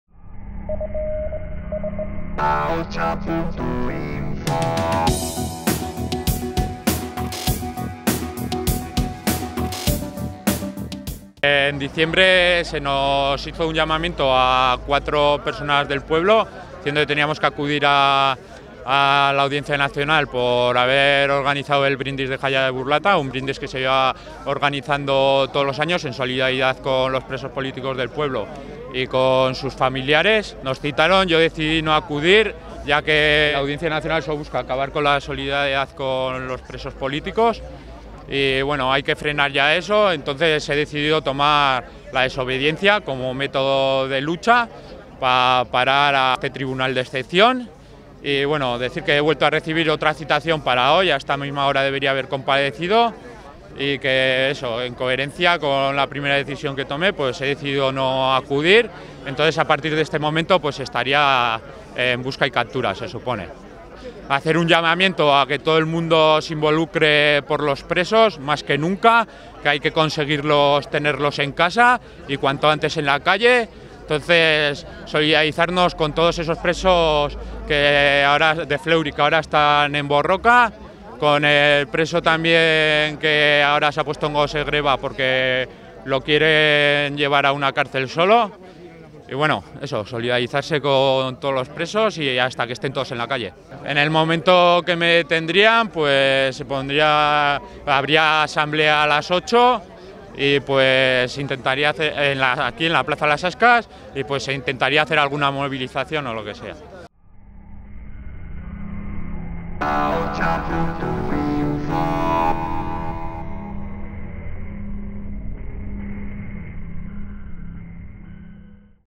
Xabier Arina desobedece a la Audiencia Nacional
El joven de Burlata Xabier Arina ha ofrecido una rueda de  prensa arropado por decenas de vecinos y vecinas de Burlata para denunciar la citación de la Audiencia Nacional por participar en un acto en favor de los derechos del EPPK. Tras no haberse presentado en la Audiencia Nacional, esta podría dar una orden de busca y captura. Si la detención se produjera, se convocarían movilizaciones de forma inmediata.